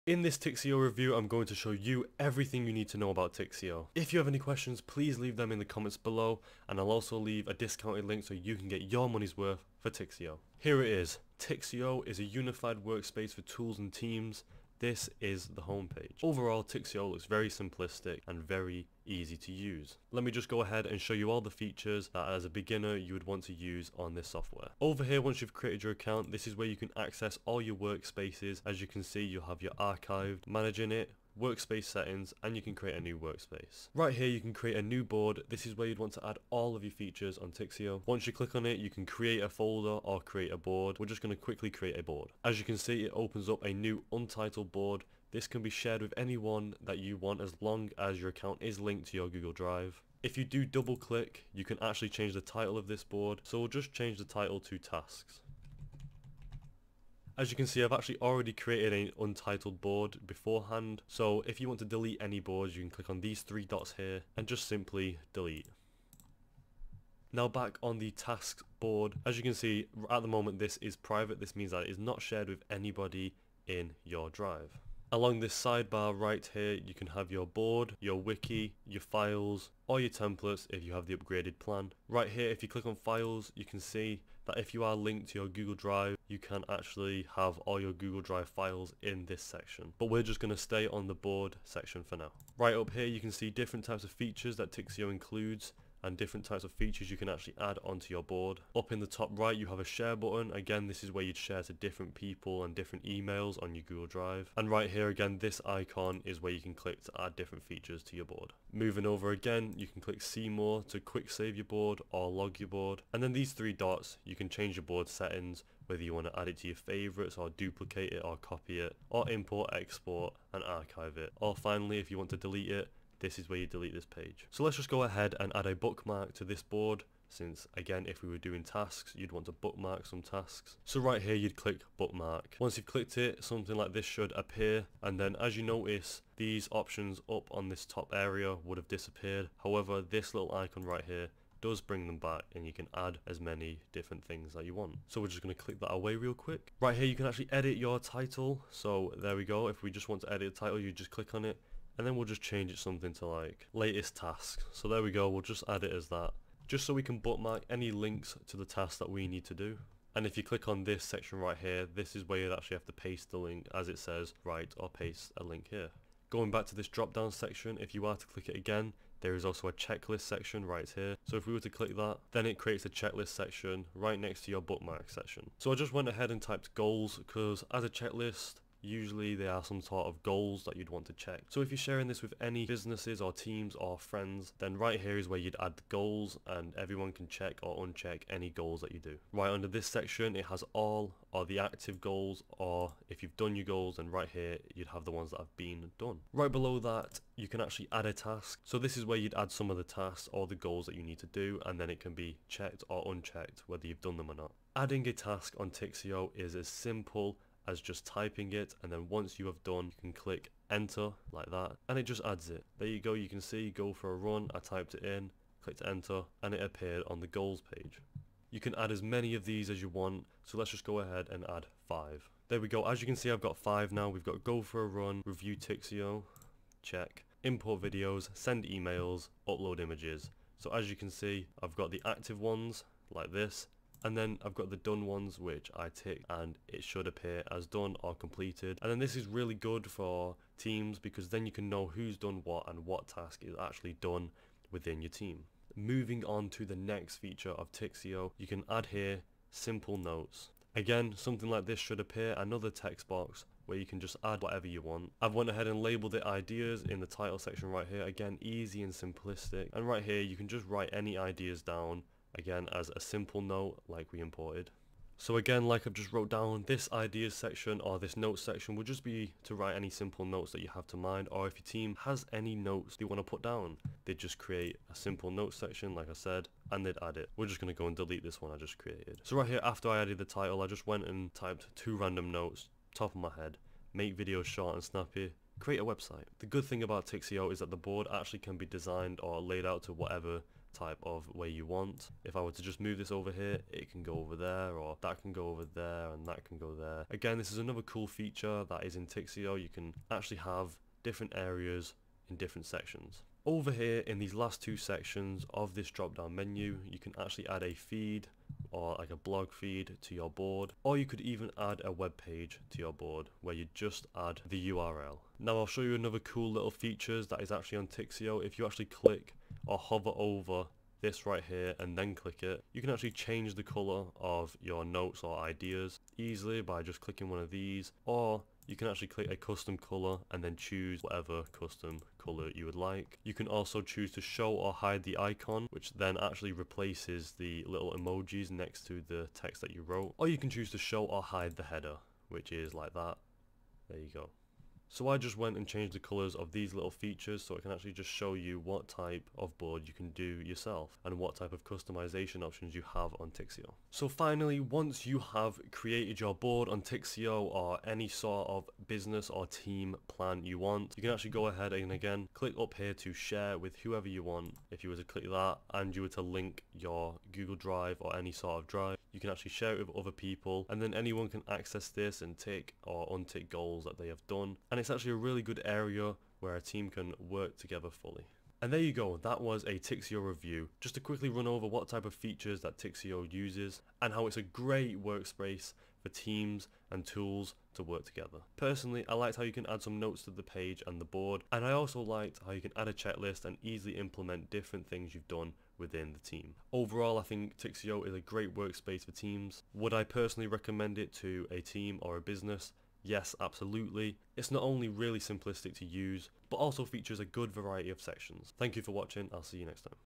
Tixio Review: Everything You Need To Know About Tixio!
Tixio Review: In this Tixio demo, I'll be walking you through creating, organize, and share information with your team from one centralized workspace.

It’s hard to get work done when your remote workplace is littered with bookmarks, files, and open tabs. (“You call it chaos, I call it the reason I’m behind my deadlines.”)

Without a system, all your work tools and data sources quickly become digital clutter that literally slow down your device and your productivity.

You need a virtual HQ that adapts to your team’s needs, so you can work and collaborate seamlessly wherever you are.

Enter Tixio.

🕐Chapters🕐
0:00 Tixio Review
0:10 Tixio Discount
0:20 Exploring Tixio's Dashboard